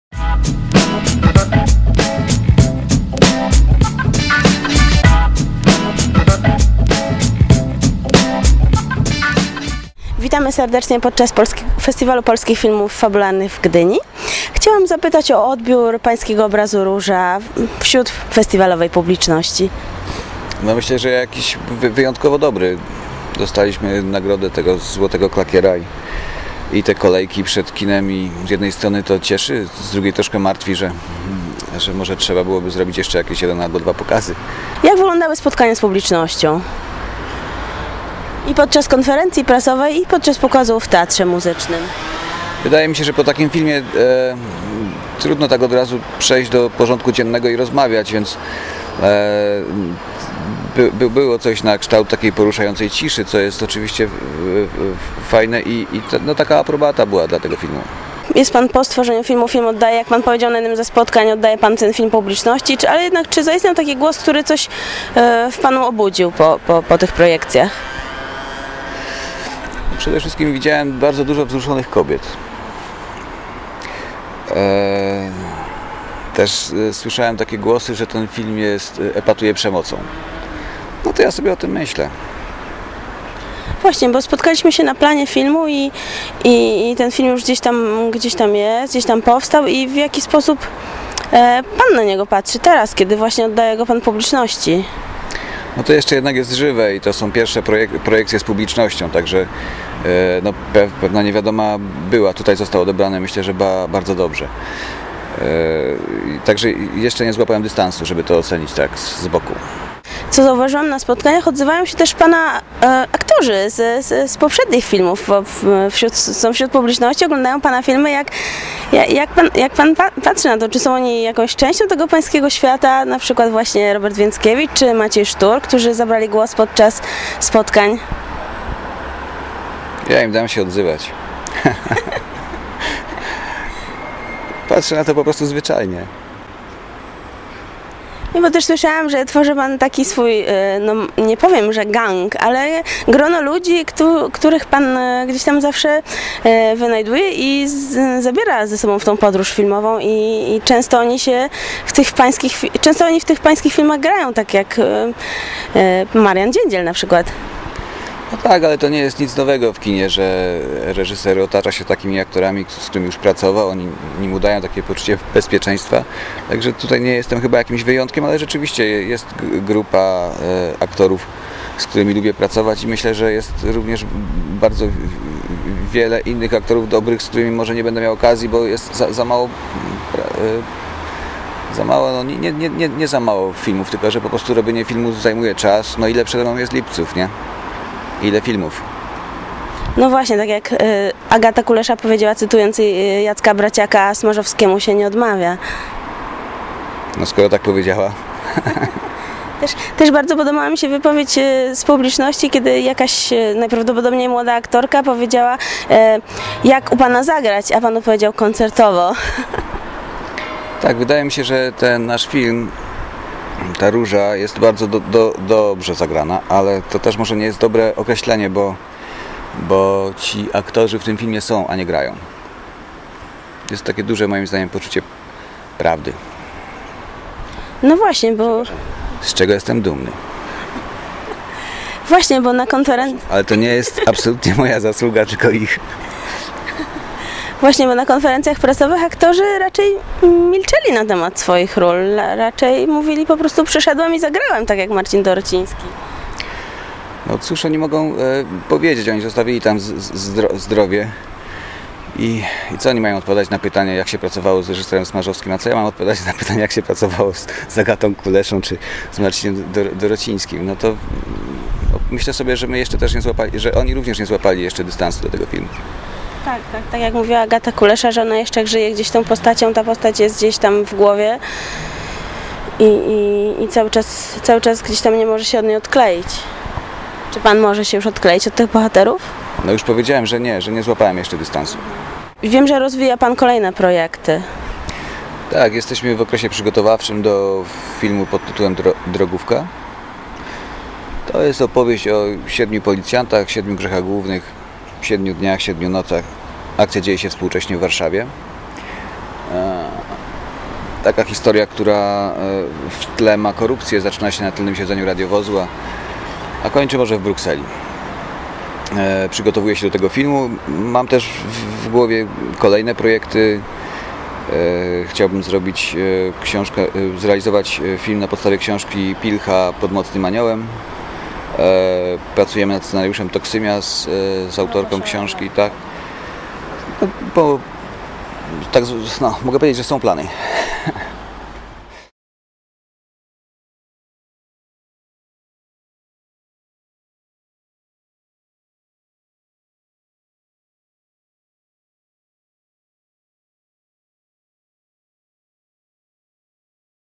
Rozmowa z Wojciechem Smarzowskim
Podczas 36. Festiwalu Polskich Filmów Fabularnych w Gdyni rozmawiamy z Wojciechem Smarzowskim, którego najnowszy film „Róża" znalazł się w Konkursie Głównym. Reżyser mówi o odbiorze swojego filmu podczas festiwalu, odpowiada na pytania dotyczące kreacji aktorskich w jego filmach oraz przybliża swoje nowe projekty.